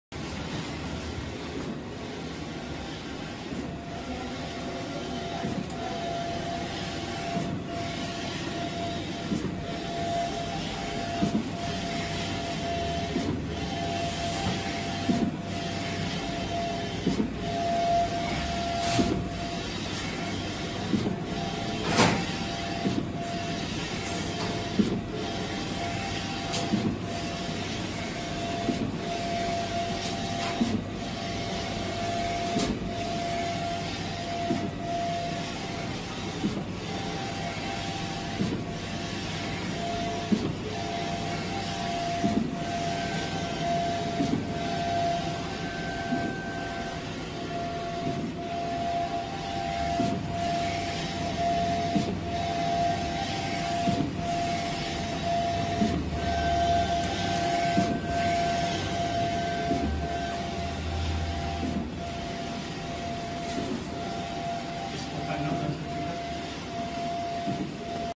Epson I3200 convertkit con la mejor calidad de impresión y un costo elevado
El cabezal de impresión Epson I3200 es rentable. Brinda alta productividad y alta calidad de imagen con una resolución de alta densidad de 600 ppp